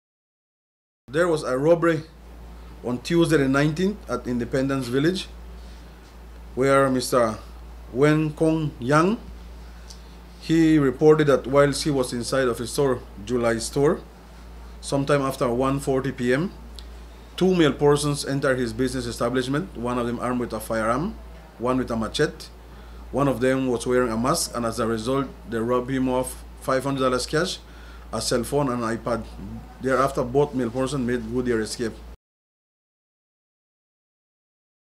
July’s Store is Robbed in Mango Creek
A store owner was robbed in Independence Village on Tuesday afternoon by a pair of thieves armed with a handgun and a machete.  Wen Quong Yian was relieved of five hundred dollars in cash, as well as a cellular phone and tablet.  Here’s more from CIB investigator Alejandro Cowo.